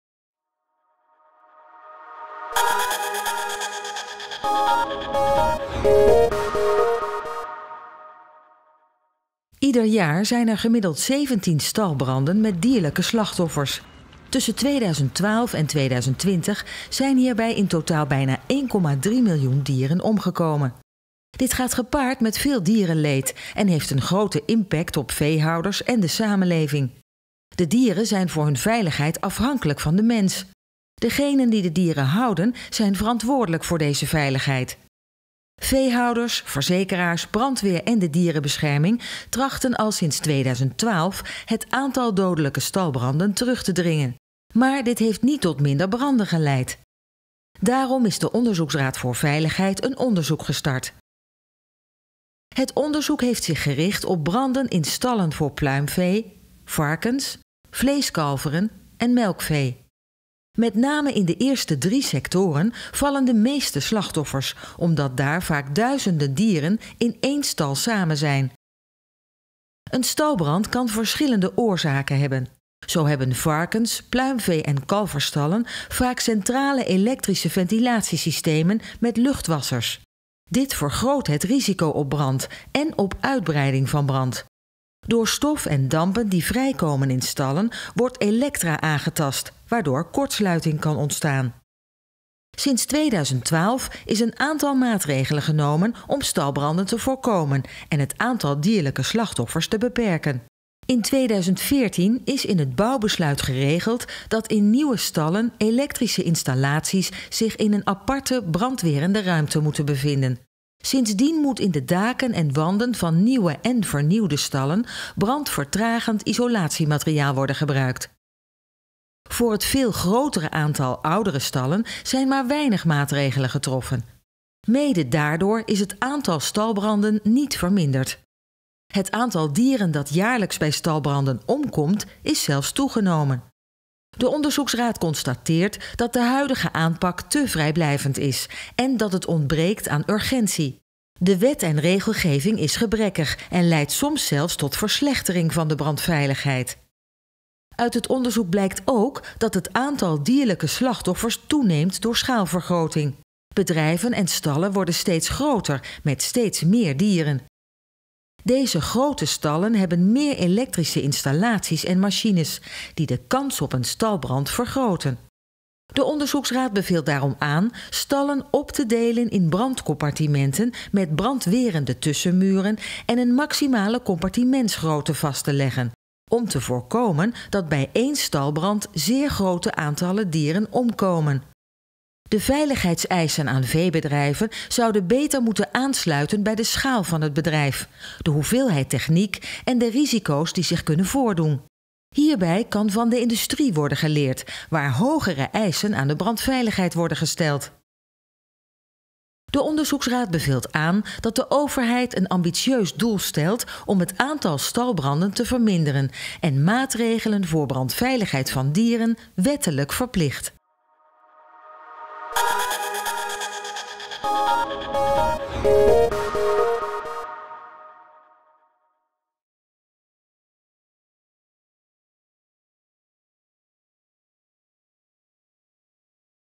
Stalbranden
De afgelopen jaren zijn honderdduizenden dieren bij stalbranden omgekomen. De gevolgen van een stalbrand zijn vaak desastreus voor dieren in die stal en zeer ingrijpend voor veehouders. De dieren verbranden levend, stikken door rook of raken ernstig gewond. Dit maakt dat stalbranden vaak emotionele reacties oproepen in de samenleving.

Omdat het erop leek dat het aantal stalbranden en het aantal dieren dat daarbij om het leven komt niet afnamen, heeft de Onderzoeksraad voor Veiligheid besloten een onderzoek naar stalbranden te doen. De Raad beoogt met dit onderzoek het aantal stalbranden, en het dierenleed als gevolg van die branden, te verminderen.